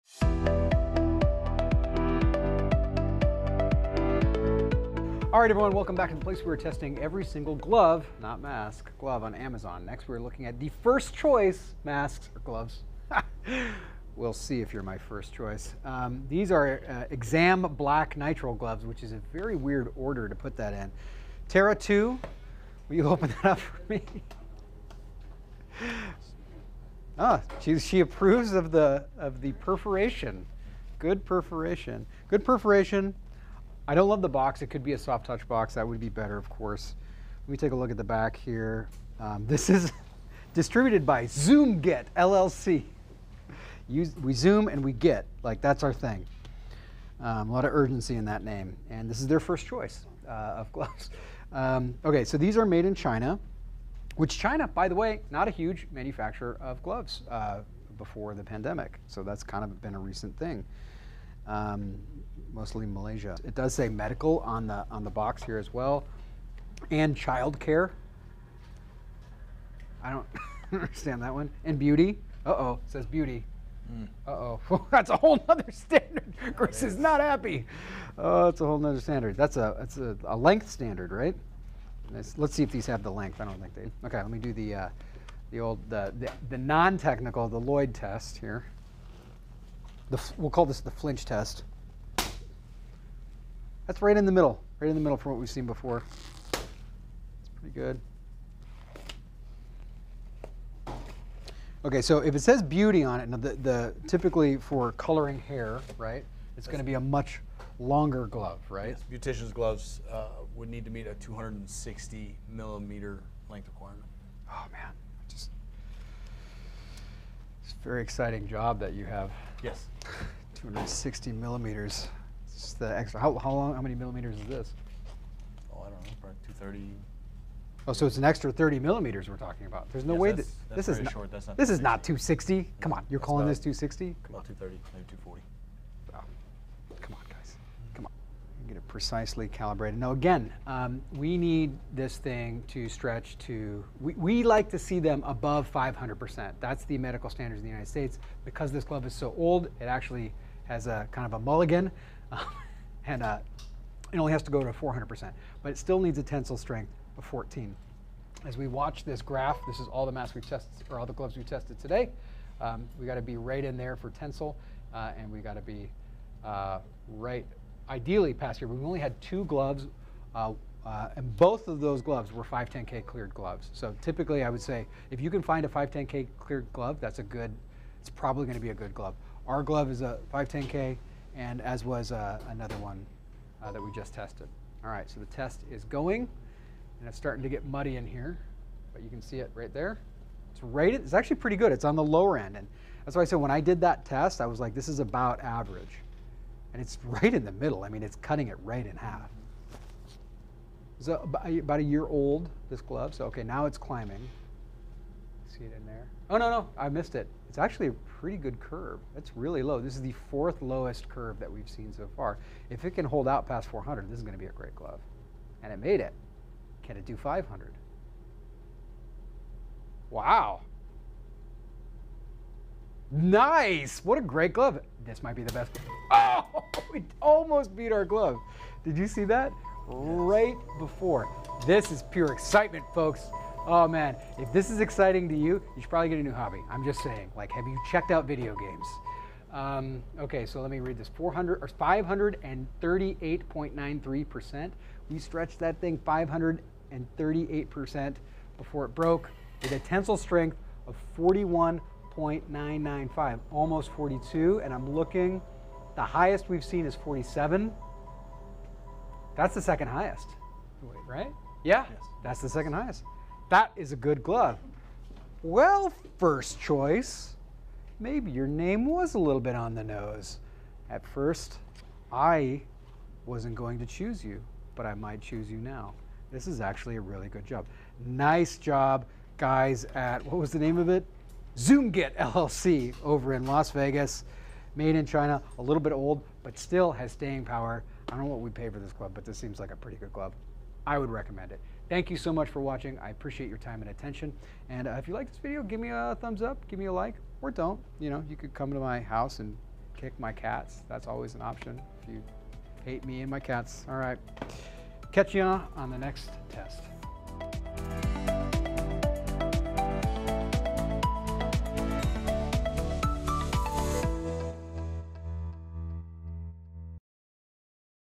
Pure Excitement - 1st Choice Exam Black Nitrile Gloves Review
We are testing every glove we can find on Amazon!  In this video,  Lloyd checks out the 1st Choice Exam Black Nitrile Gloves manufactured in China.